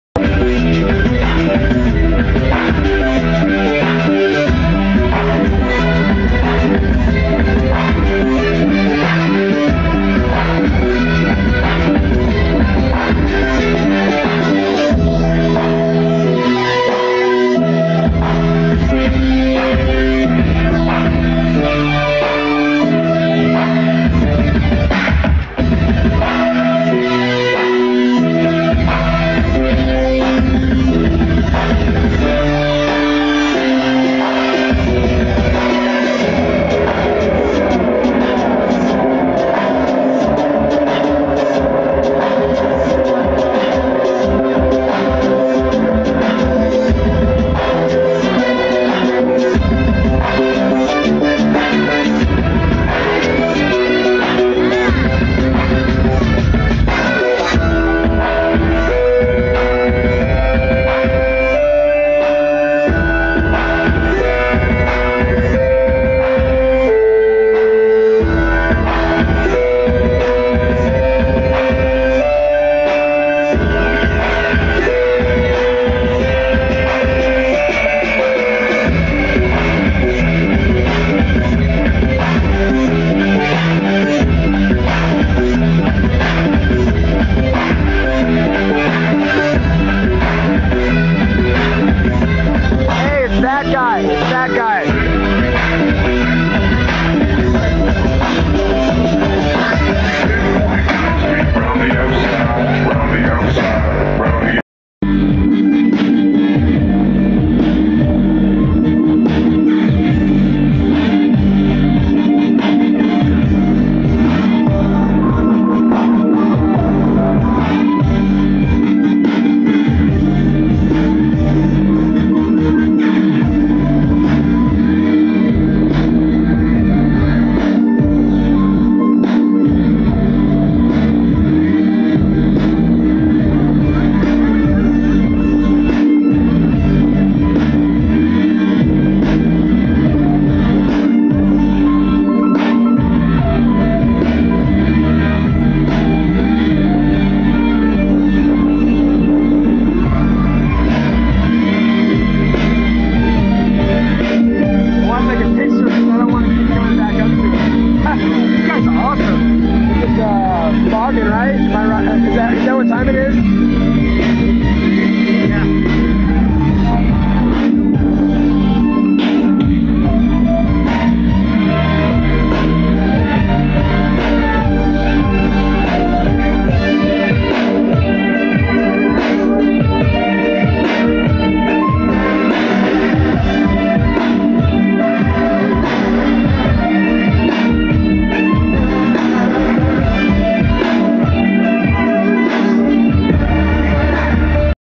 Stilldream 11 - Boggan
*~* Dorset Perceptions Festival Videos *~* This was our first time seeing DJ Boggan and he absolutely ripped it apart! Really crunchy, industrial dubstep and midtempo type of stuff. You rock Boggan, and your hair is amazing! *~* Also check out